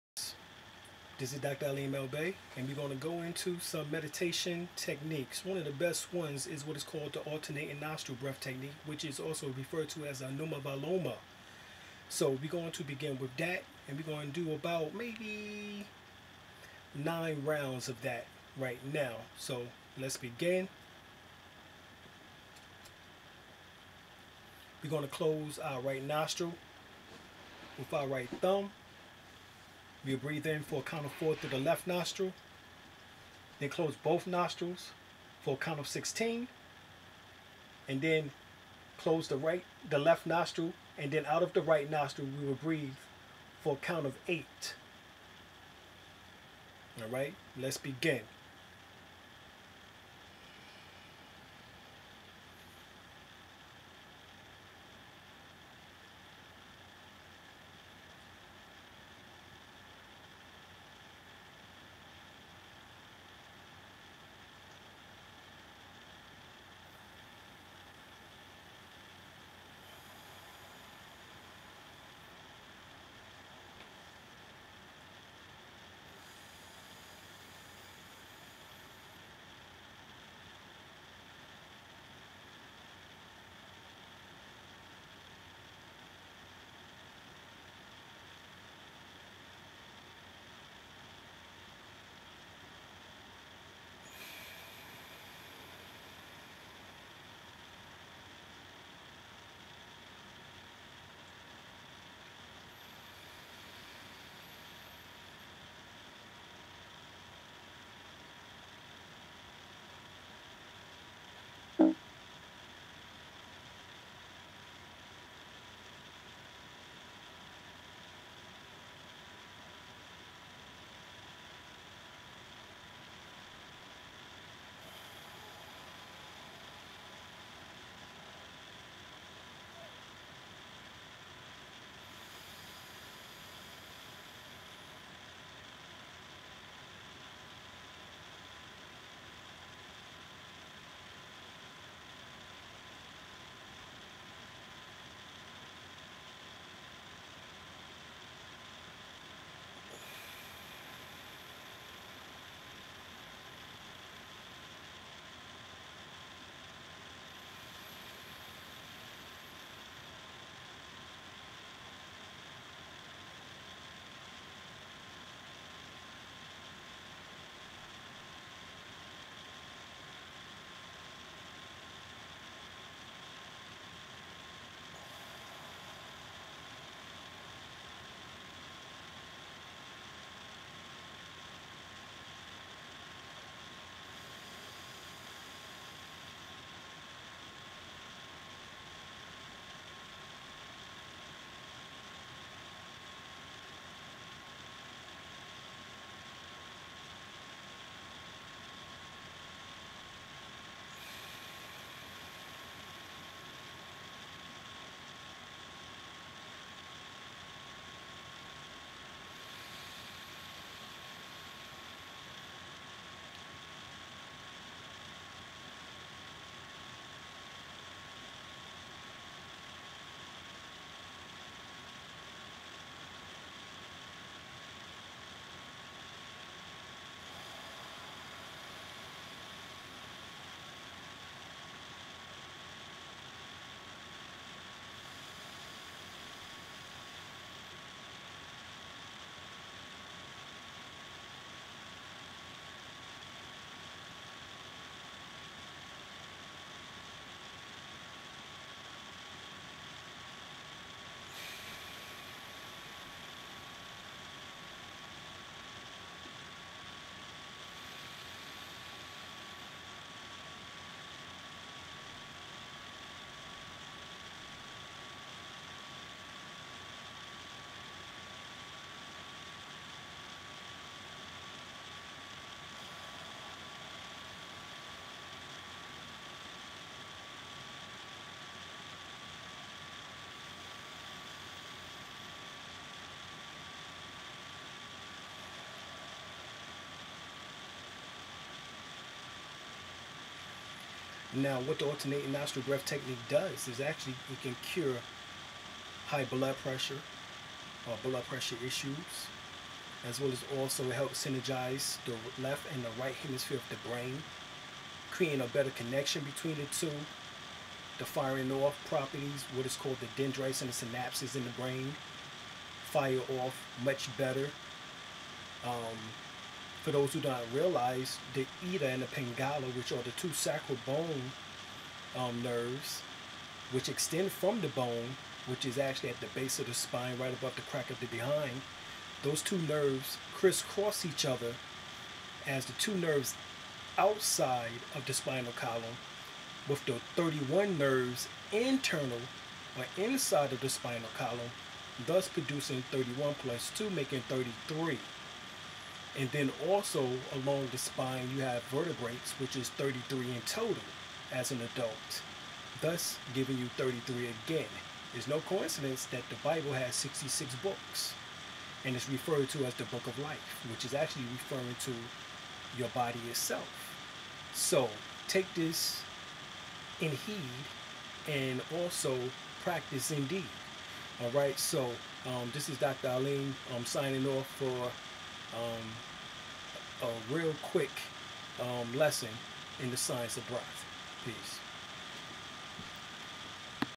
Dr. Alim El-Bey: "ONE OF THE MOST POWERFUL BREATH TECHNIQUE!"
The Most Powerful Breath Technique this 5 minute video takes you through the Anumaviloma which is the name of the Alternating nostril technique.  This technique synergies' both hemispheres of the brain relieves high blood pressure and improves circulation.  Practice with us everyday at least once a day.  You deserve to mediate.